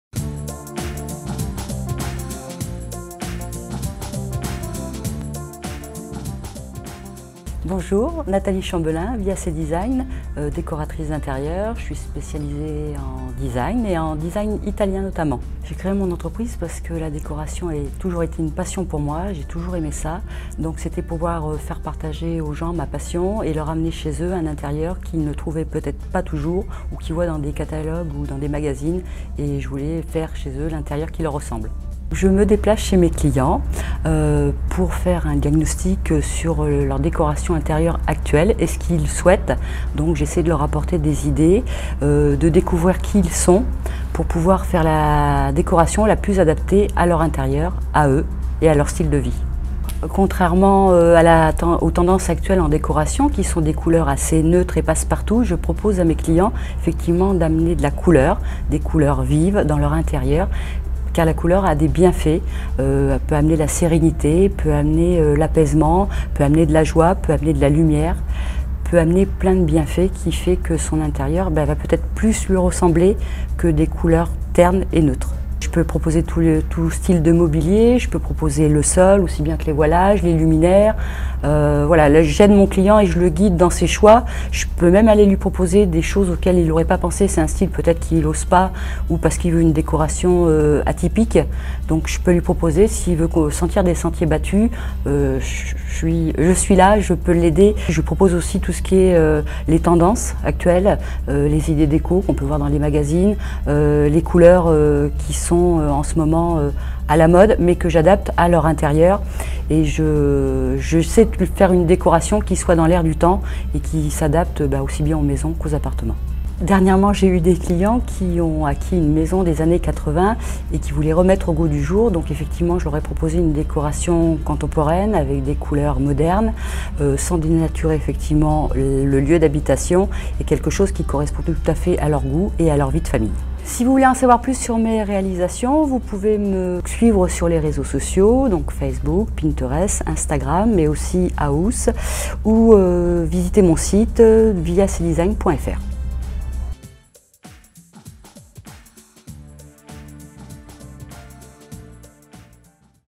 VIACDESIGN, lauréate des Rencontres Jeunes Entreprises (15.06.2016)
Viacdesign est l'une des 10 entreprises gagnantes d'un clip vidéo de présentation offert par la CCI de Bordeaux lors des Grandes Rencontres Jeunes Entreprises du 15 juin 2016.
Ces rencontres font partie d'un programme de RDV d'affaires organisés sur la Gironde par la CCI de Bordeaux pour accompagner le développement des jeunes pousses